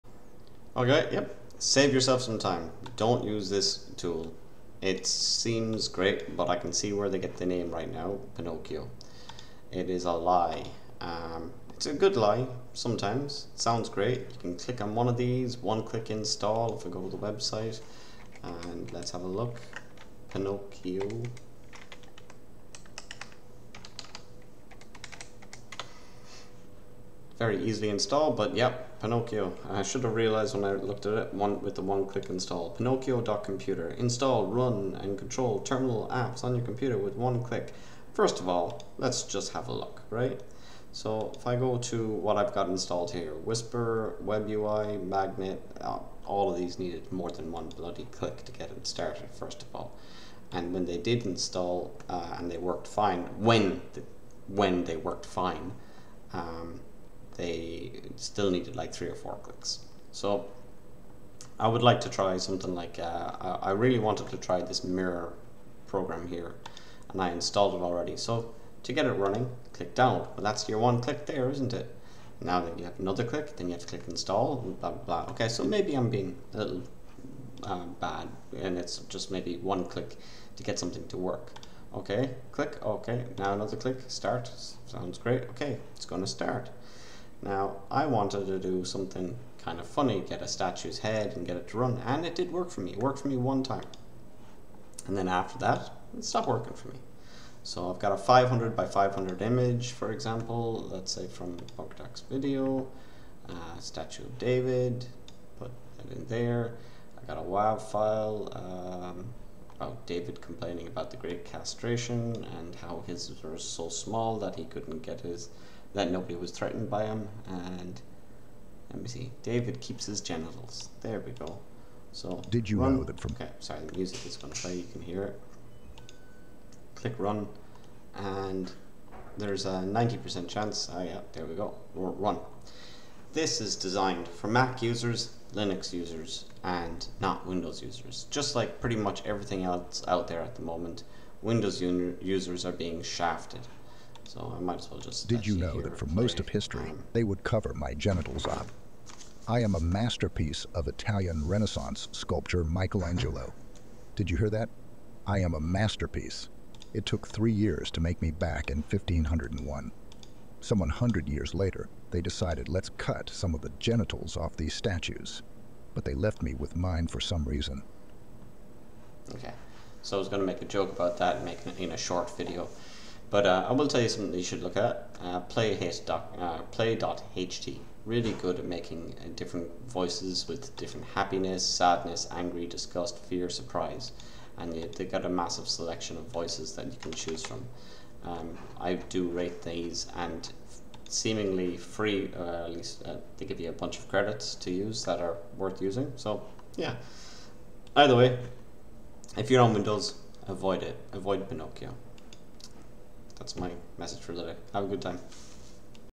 Windows Warning | Pinokio.computer | Fail
Pinokio.computer is designed for Mac machines no matte what they say. It does not easily work on windows. Pinokio.computer is an amazing tool and I wish it did work for me but I found that it is too difficult to use as a handy tool as you spend a lot of time debugging pinokio.computer.

I laughed when I realised the logo for pinokio.computer is pinokio and they say it is a one click install.
It is like they are misleading to your face.
However cool Pinokio.computer is, set up each tool individually yourself for better success.